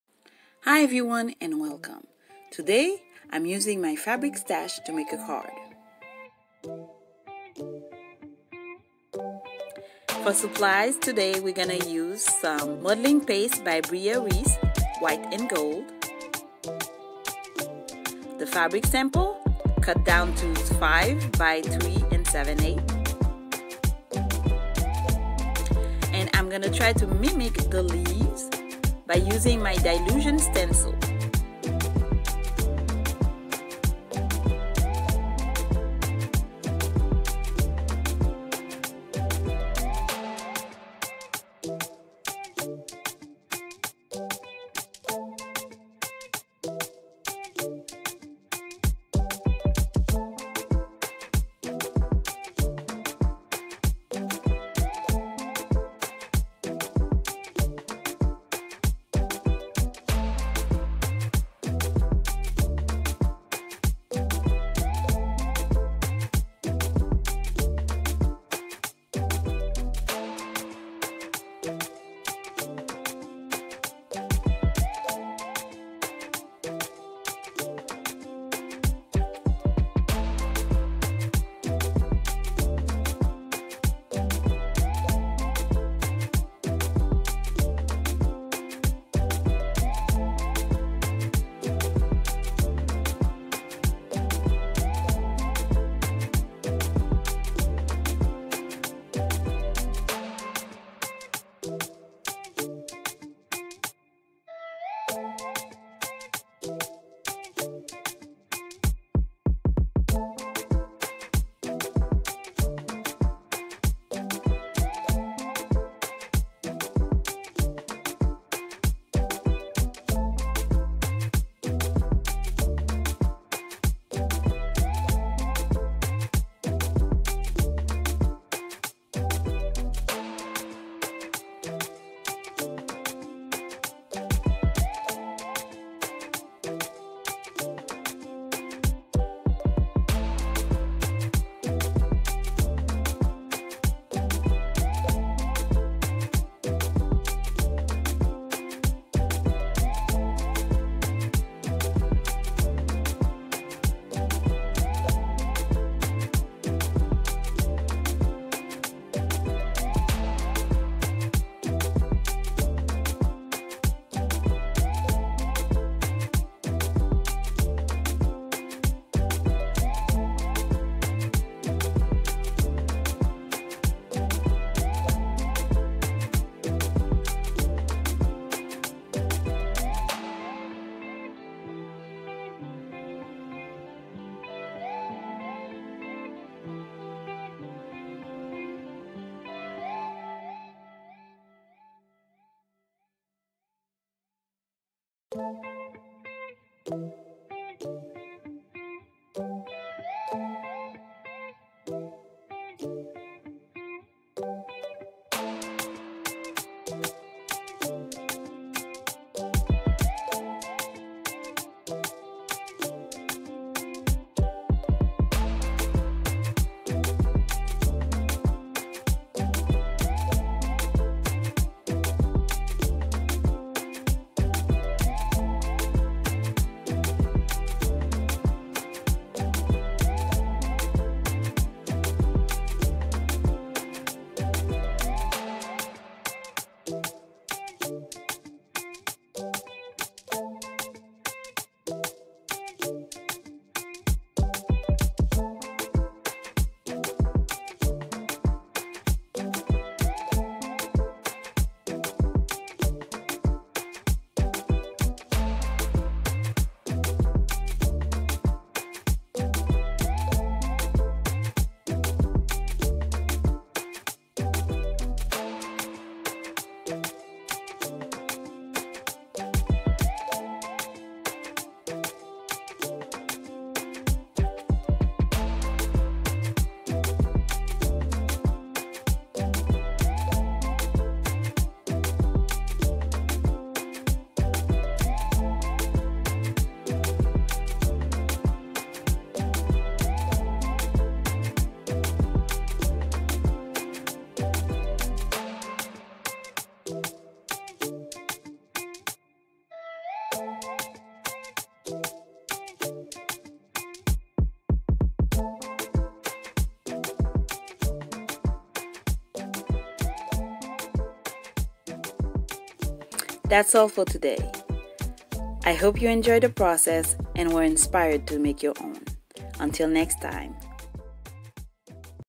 Unlikely use of fabric / Utilisation improbable d'un tissu
Hi everyone. I hope you're doing well. Today we are using a piece of upholstery fabric sample to make a card. After some bells and whistles, you will have a beautiful card. Follow my lead and put your leftover fabric to work.

Salut à tous. J'espère que vous allez bien. Aujourd'hui, nous utilisons un échantillon de tissu d'ameublement pour fabriquer une carte. Après quelques petites modification, vous aurez une belle carte. Suivez mon exemple et mettez vos surplus de tissus au travail.